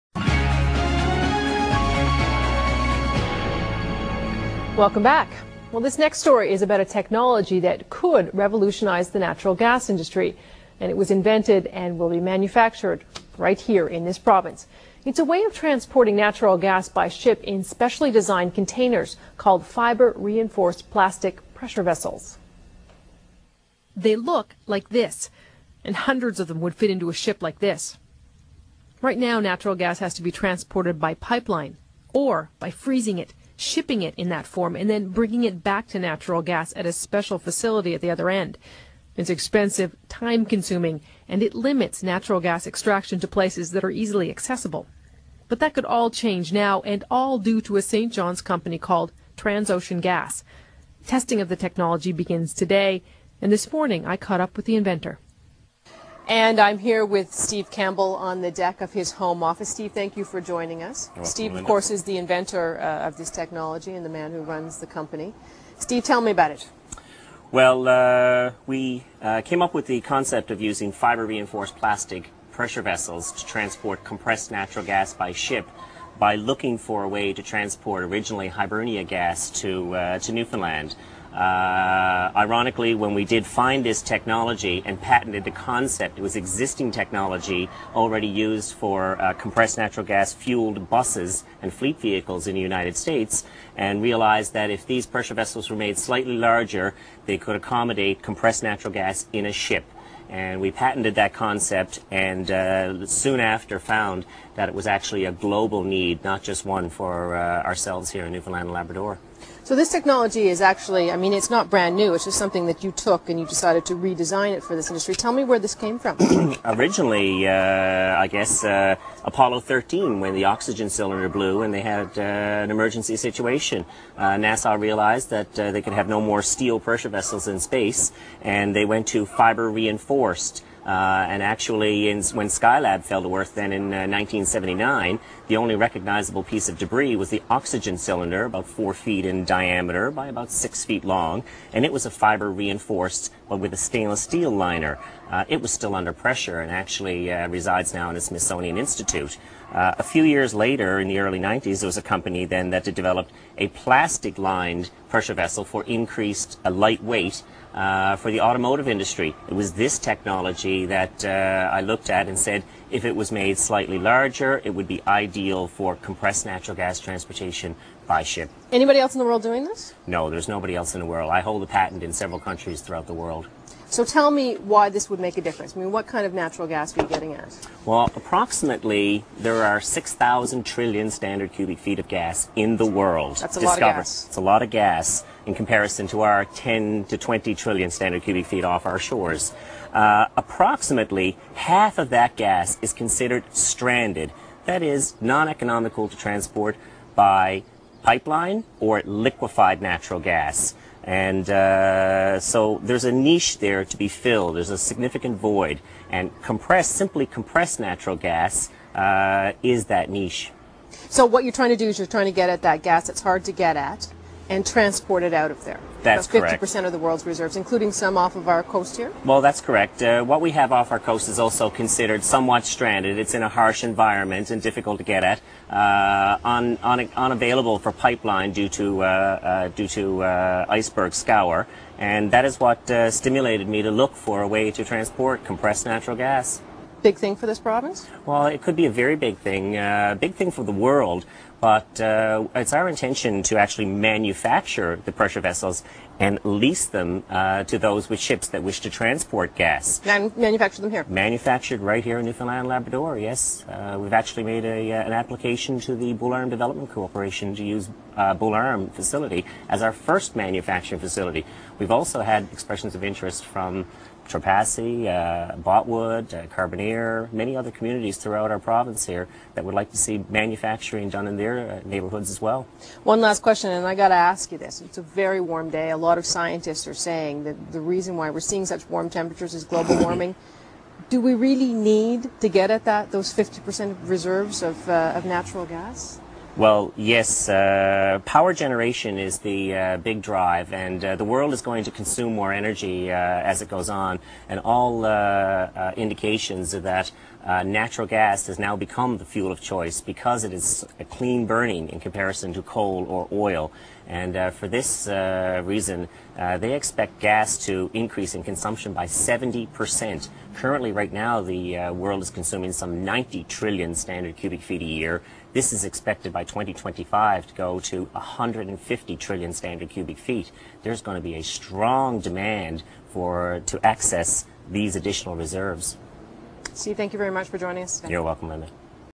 Interview with Steven Campbell of Trans Ocean gas
Interview with Steven Campbell of Trans Ocean gas about the Trans Ocean Gas, Fiber Reinforced Plastic CNG Containment System.